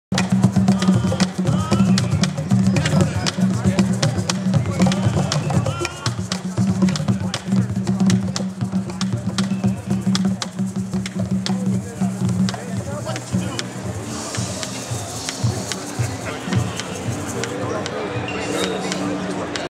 Mr. Remo Belli @ PASIC
A side of Remo we rarely get to see - Mr. Belli participating in a drum circle at PAS.
The sign reads "Shhhh - we're drumming!"